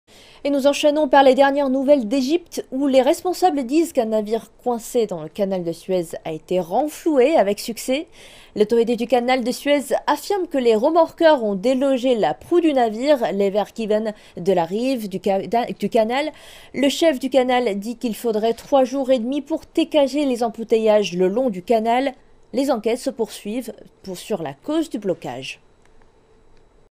Égypte : le porte-conteneurs a été renfloué
En Égypte, les responsables disent qu'un navire coincé dans le canal de Suez a été renfloué avec
succès.